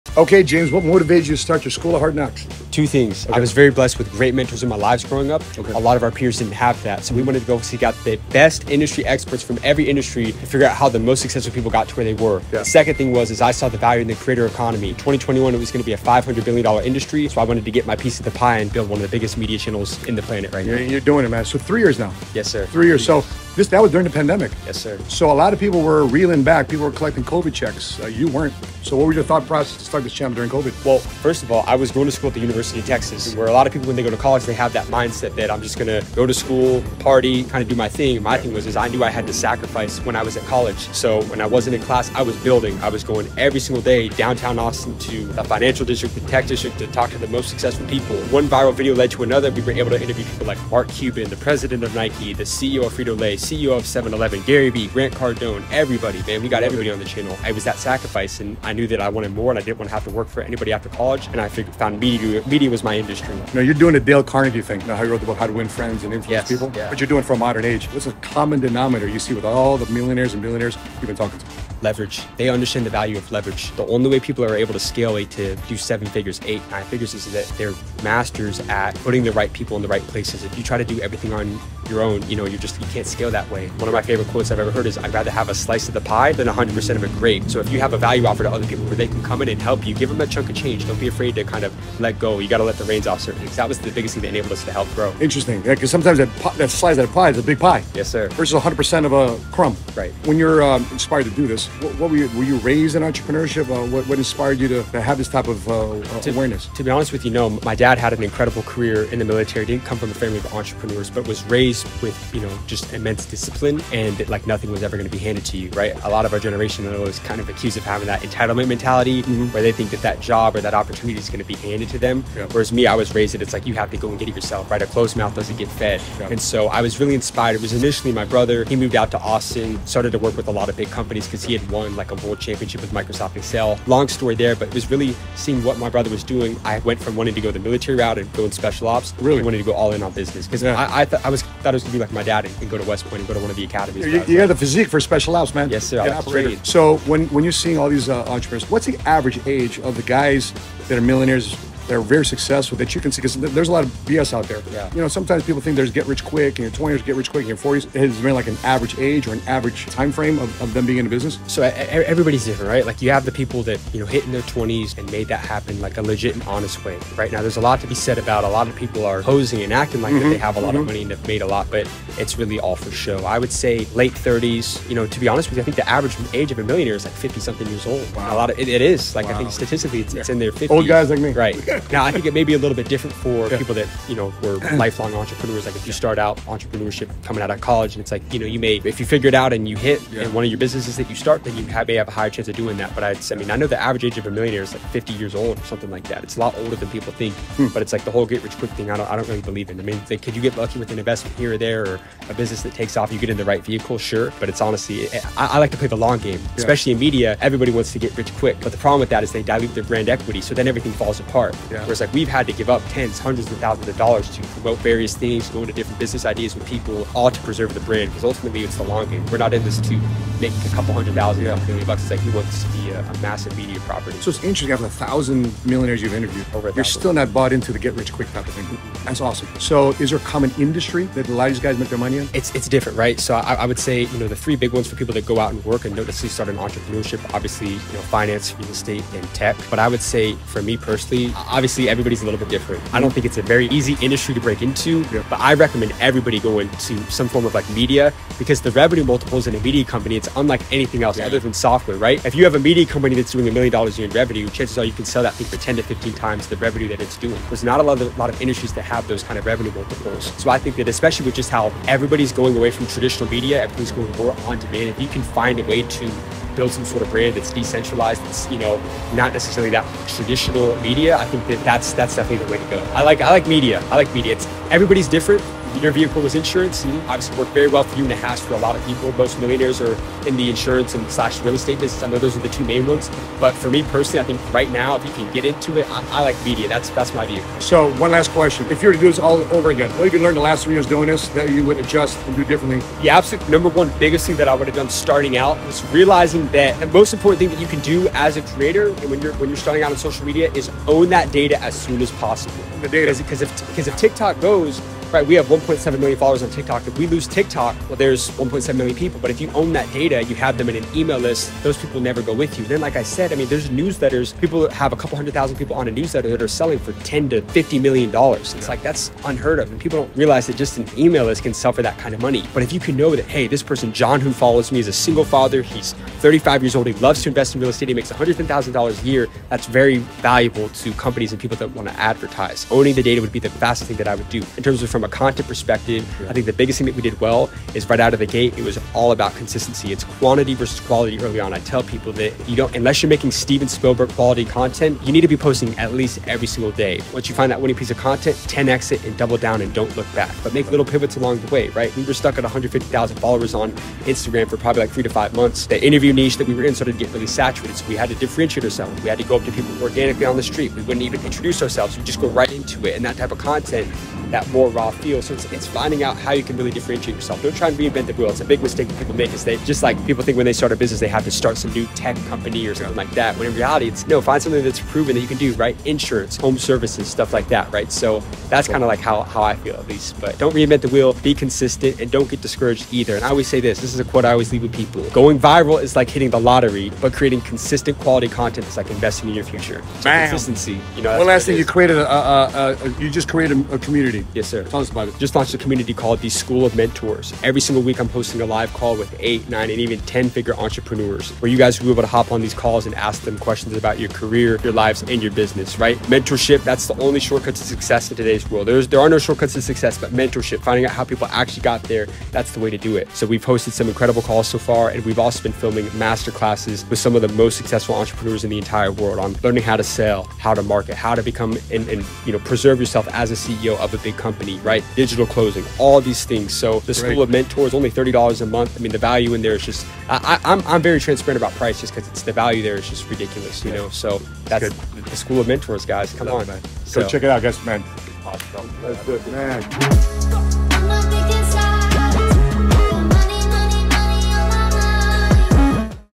Inside James Dumoulin’s Millionaire Mindset
In this clip from 7 Figure Squad, I flipped the script on James when he rolled up on me in Dallas. I get inside James Dumolin's mindset so let's learn from the mastermind behind @theschoolofhardknocks

If you want to see my interview with James on the School of Hard Knocks, here's the link starting @ 10:05

Quick shout out to Joseph James who helped make this connection